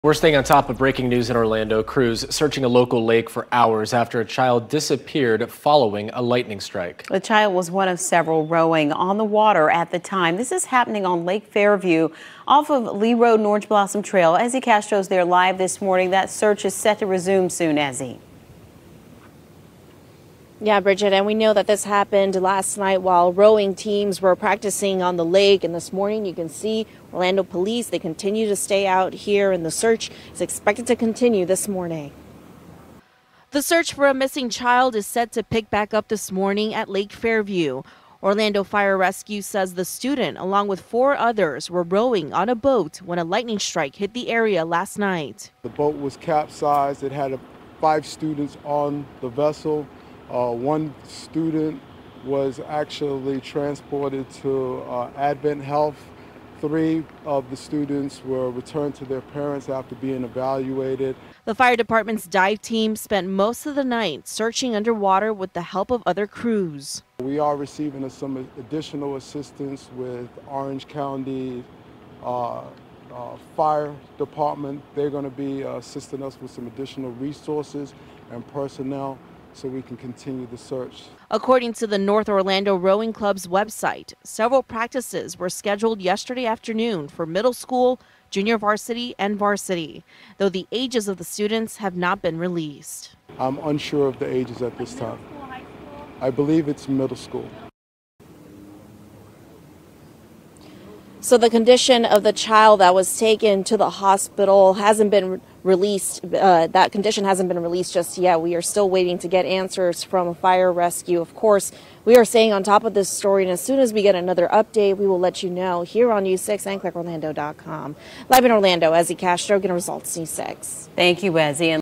Child rowing on lake disappears following lightning strike
A child believed to be a middle school student is missing on Lake Fairview in Orlando.